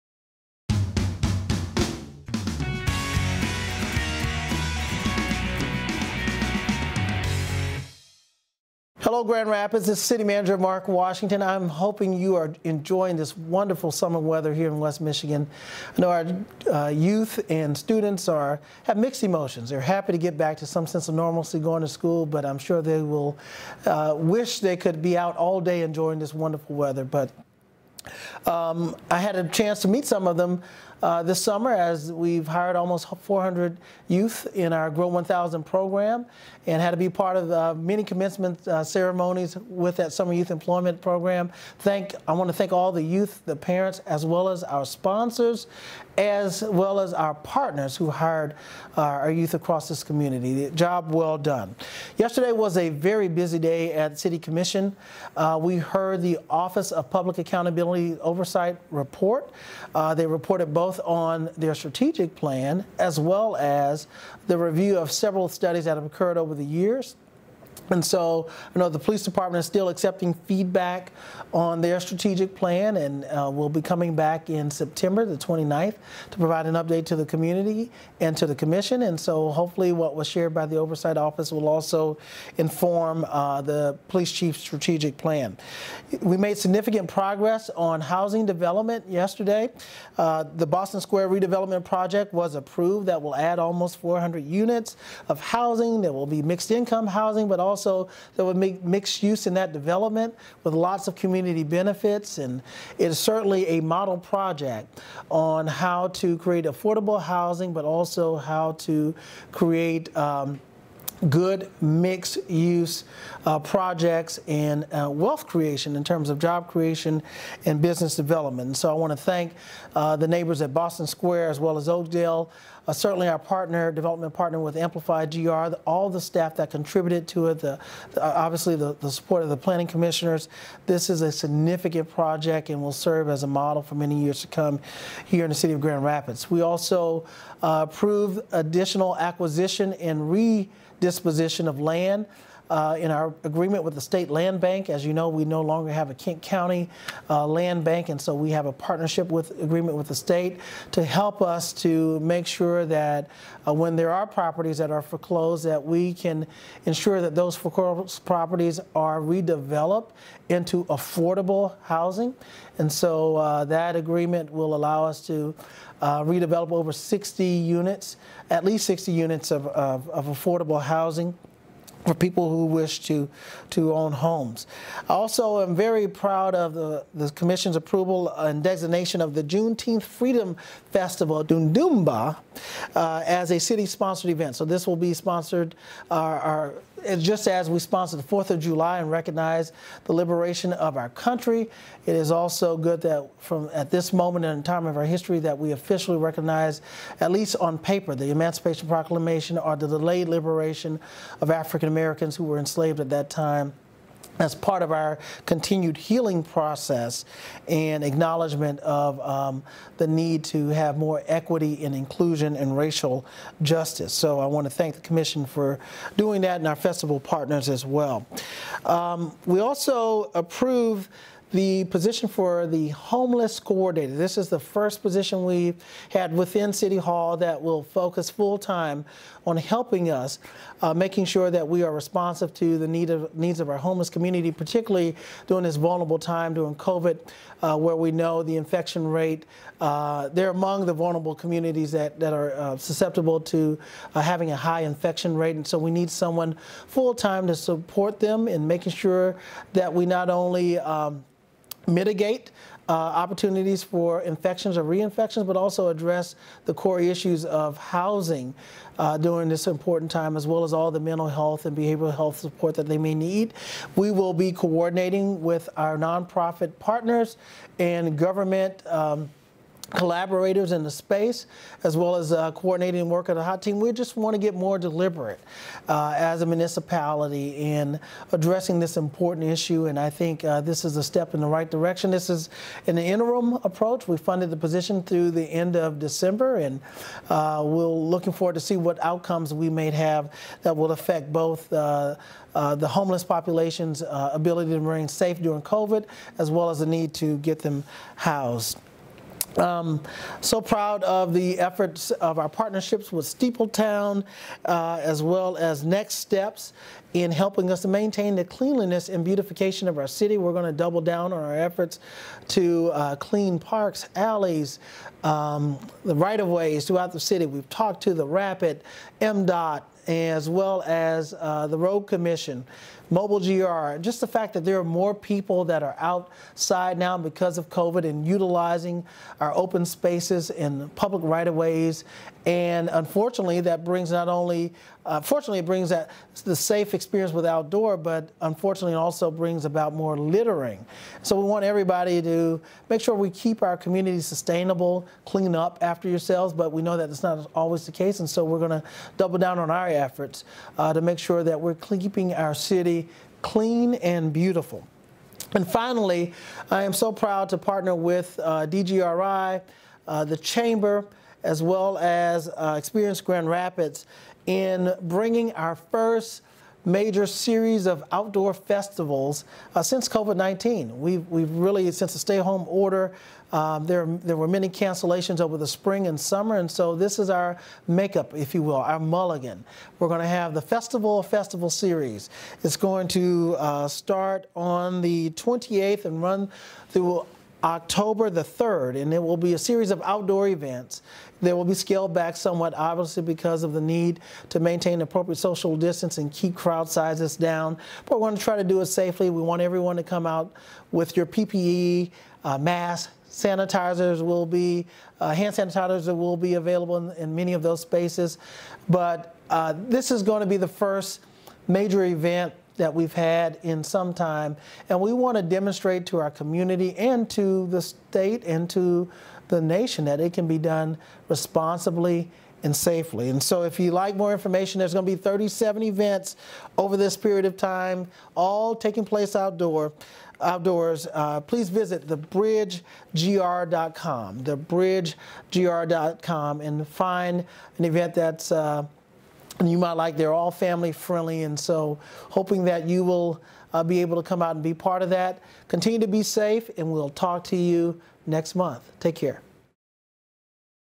City Manager Mark Washington Vlog - August 2020
In his latest vlog, City Manager Mark Washington talks about our GRow 1000 youth employment program, The Bridge GR event series and this week's City Commission meetings, which included an update from our Office of Oversight and Public Accountability - Grand Rapids and action on a community redevelopment project, housing affordability, Juneteenth and a homelessness coordinator position, among others.